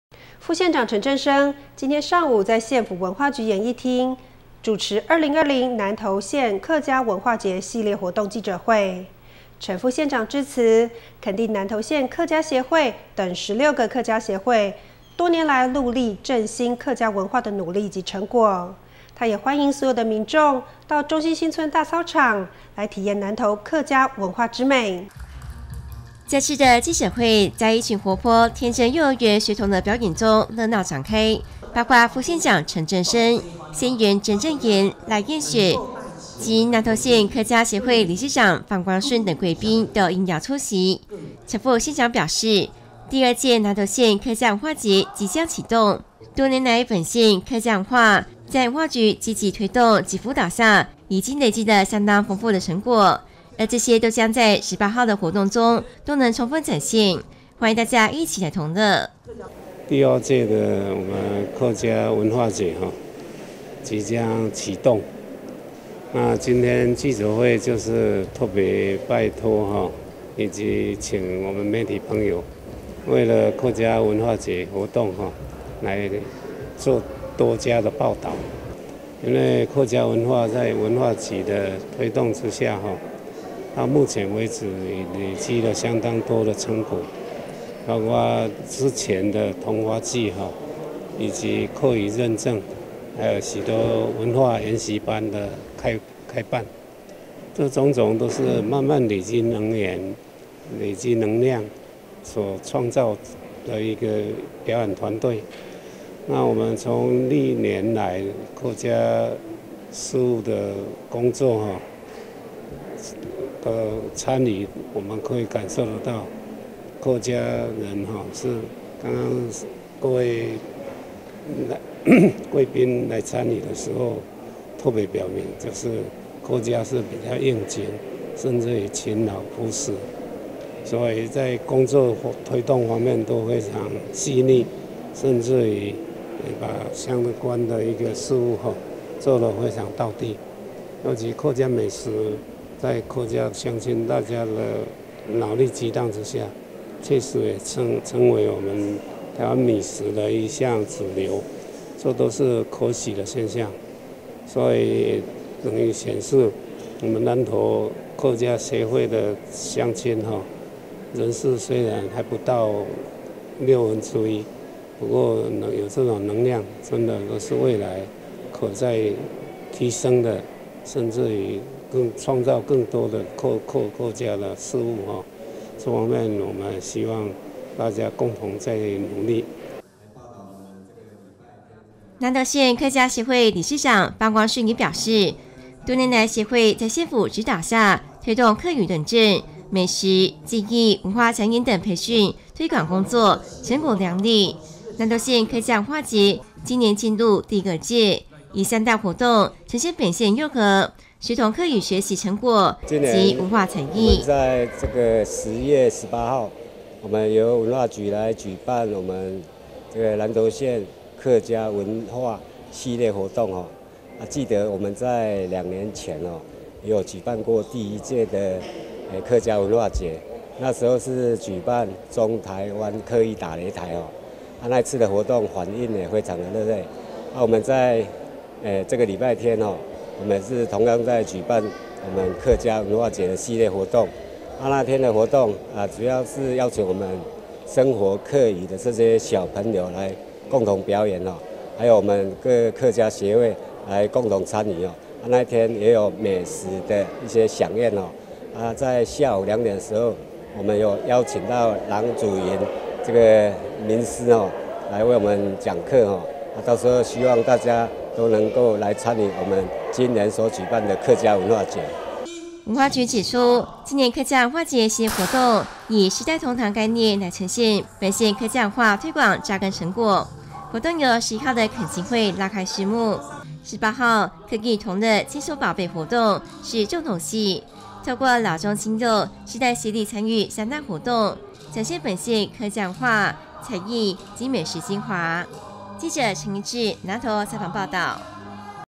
客藝同樂 牽手寶貝 活動18日精采登場
副縣長陳正昇今天上午在縣府文化局演藝廳大廳主持「2020南投縣客家文化節系列活動記者會，陳副縣長致詞肯定南投縣客家協會等16個客家協會多年來戮力振興客家文化的努力及成果，他也還迎所有的民眾到中興新村大操場來體驗南投客家文化之美。